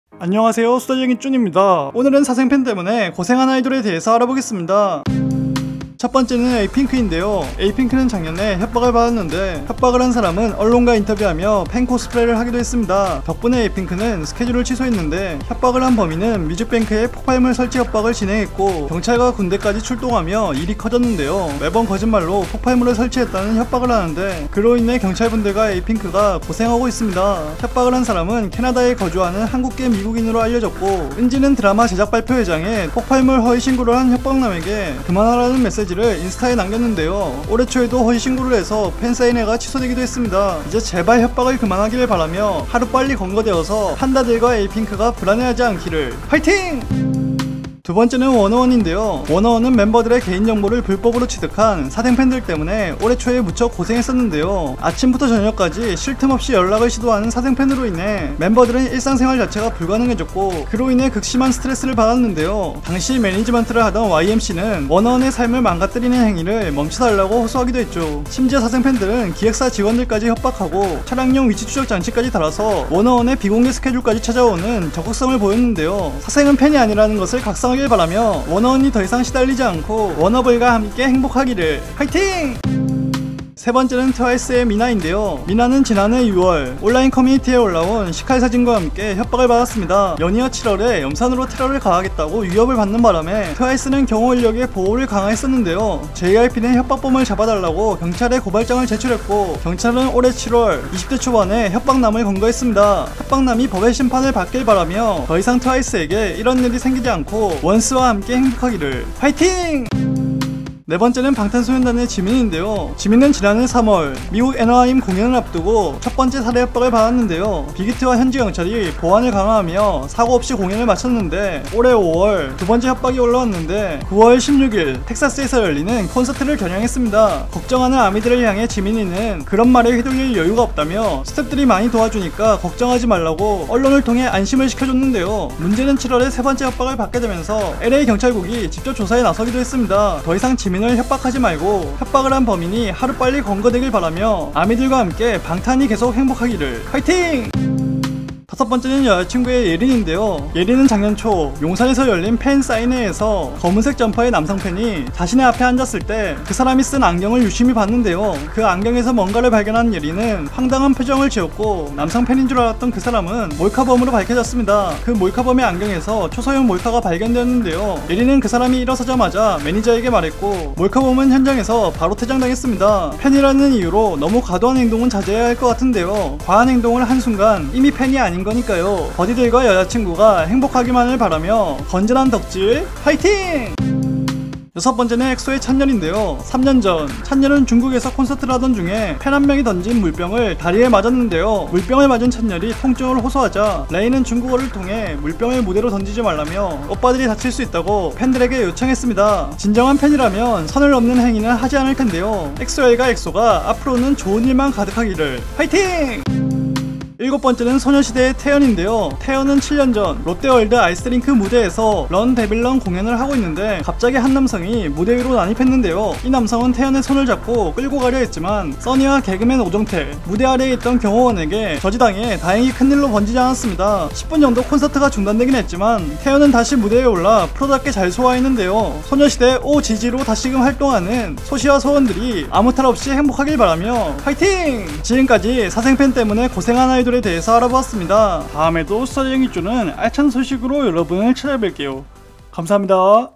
사생팬(협박범) 때문에 고생한 아이돌7 (꼭 그래야만 했냐ㅠㅠ)
오늘은 사생팬(협박범) 때문에 고생한 아이돌에 대해서 알아보겠습니다. '좋아요'와 '구독'은 콘텐츠 제작에 큰 힘이 됩니다 : )
(This video includes English subtitles - Turn on Youtube ENG CC)

2017 ⓒ SUDA JJ's K-POP Entertainment.